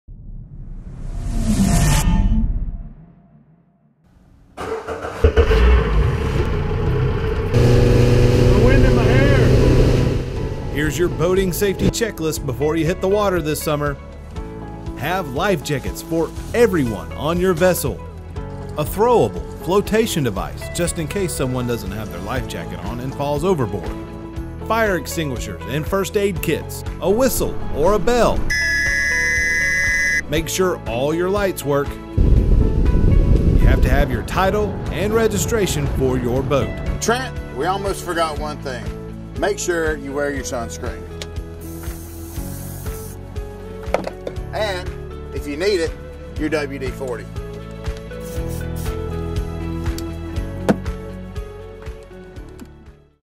YCSO Lake Patrol Safety Check List
Here's a quick boating safety check list video as we head into the MemorialDay holiday weekend.
to learn more about our agency.